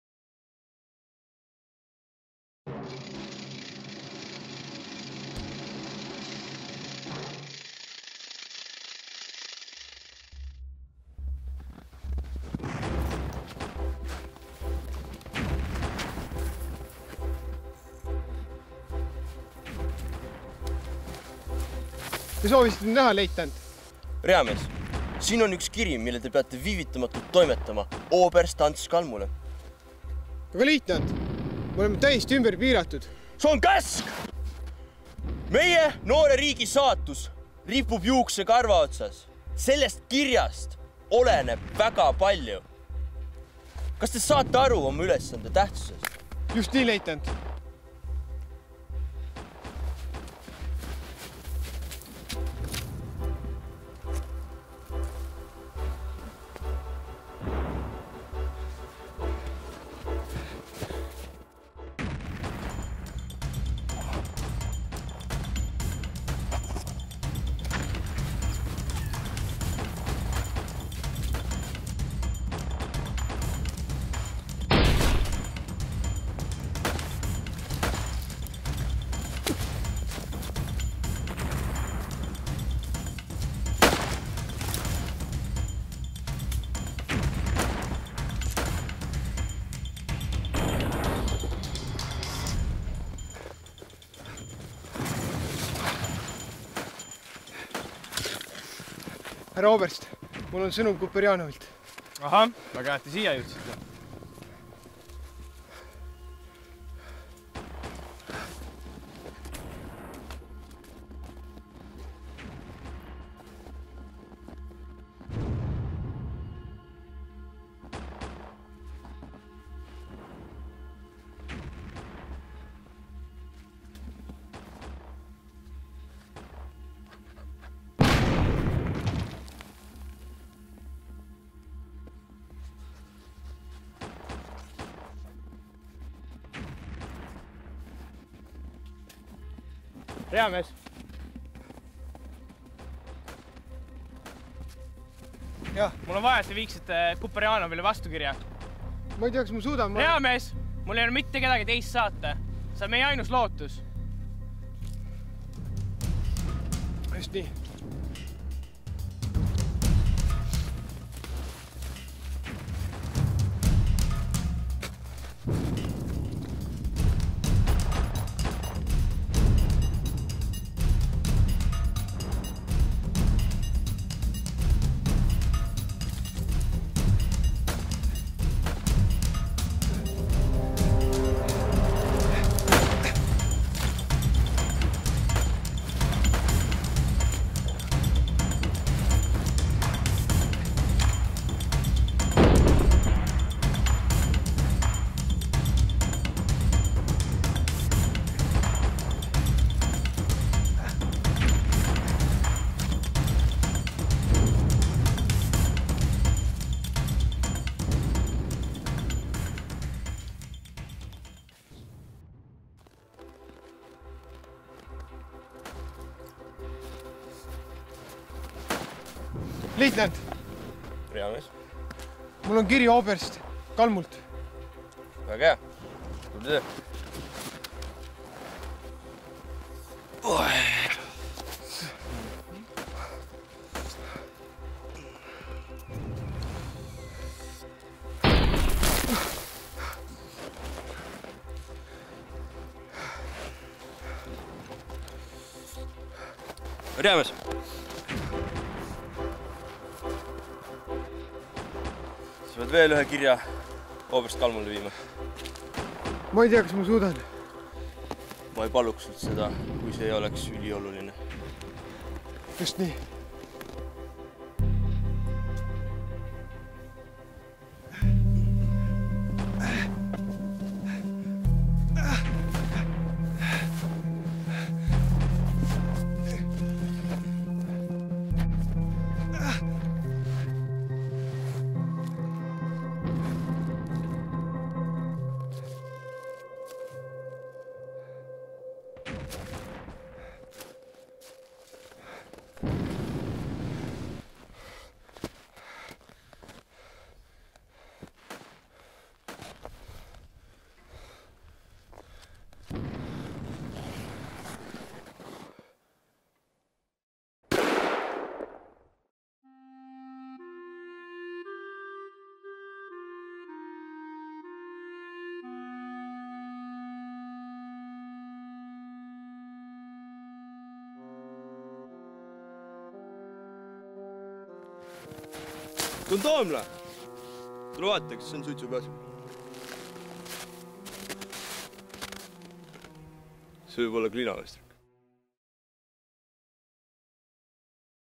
Kuperjanovi surm [NUZZ Filmitiim]
Ajalooürikud räägivad, et leitnant Julius Kuperjanov sai surma juhtides jalaväe rünnakut Paju mõisale - kuid meil on sündmustest oma nägemus.